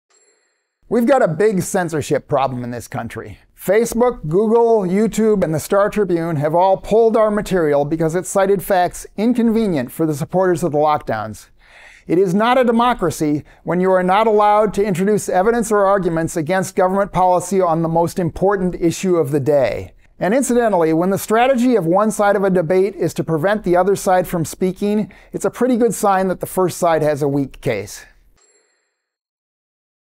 McTavish: We've got a big censorship problem.
We’ve got a big censorship problem in this country.  Facebook, Google, Youtube, and the StarTribune have all pulled our material because it cited facts inconvenient for the supporters of the lockdowns.  It is not a democracy when you are not allowed to introduce evidence or arguments against government policy on the most important issue of the day.  And incidentally, when the strategy of one side of a debate is to prevent the other side from speaking, it is a pretty good sign that the first side has a weak case.